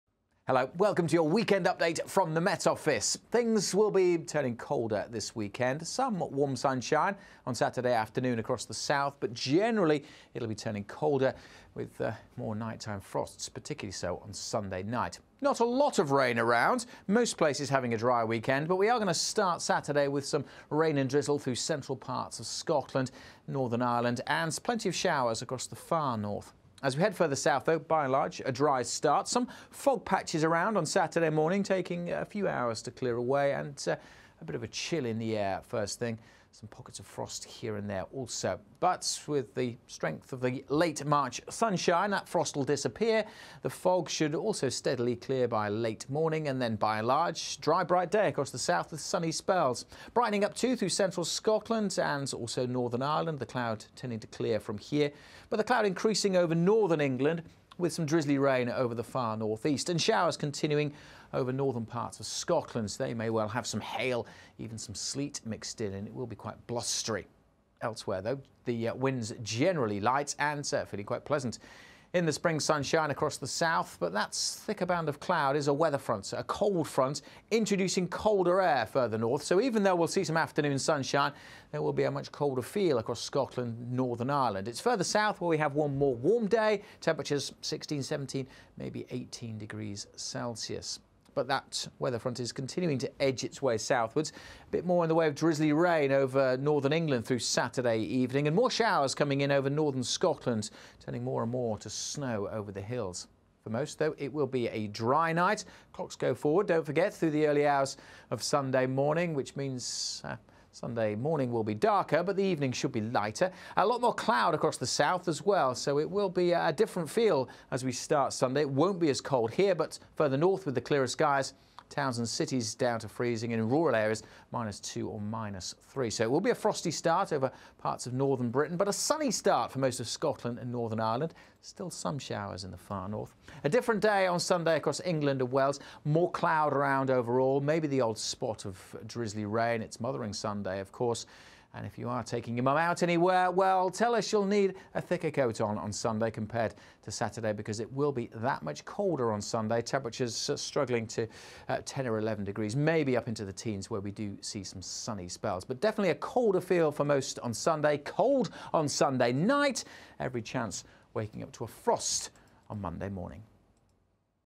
Saturday morning forecast 30/03/19
30 March weather forecast presented by Alex